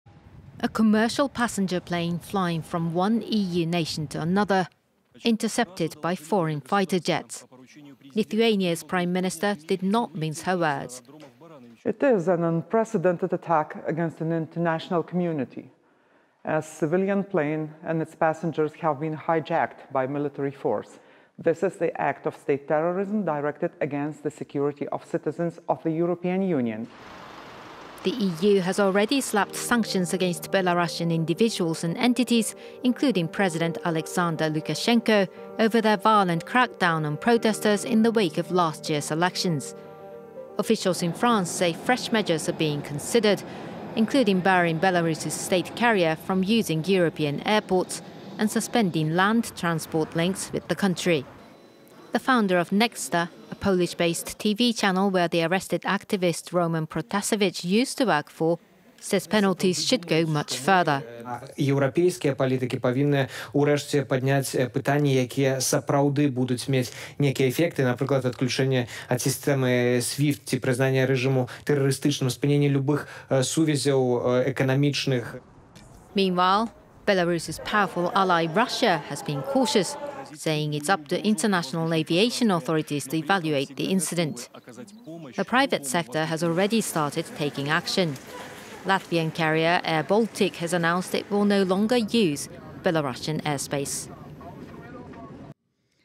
Belarus plane diversion an ‘act of state terrorism’ Lithuanian PM says
Anger over the forced landing of a Ryanair plane in Belarus is overtaking today’s EU summit in Brussels. That plane was travelling from Athens, Greece to Vilnius, Lithuania when Belarusian authorities flagged what turned out to be a false bomb alert, forcing the plane to land in Minsk. Once on the ground, authorities arrested an opposition activist, who was among the passengers.